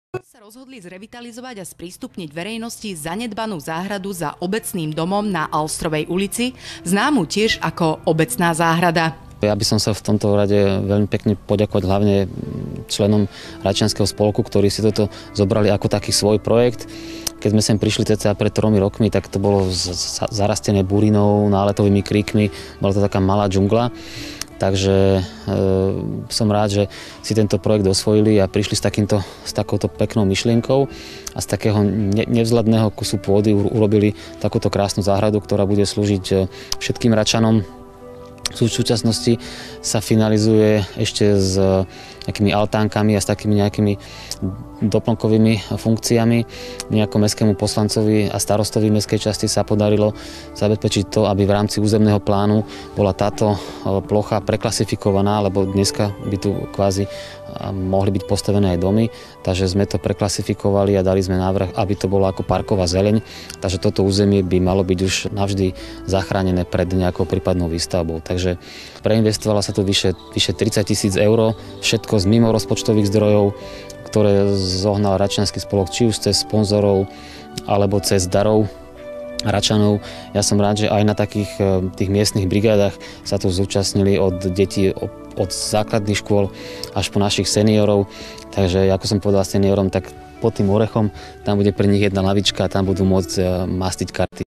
Obecná záhrada v Rači
Starosta Mestskej časti Rača Peter Pilinský ďakuje členom Račianskeho spolku, sympatizantom, darcom a brigádnikom za revitalizáciu Obecnej záhrady na Alstrovej ulici.
Zdroj: Račiansky magazín, 11/2014
TV Bratislava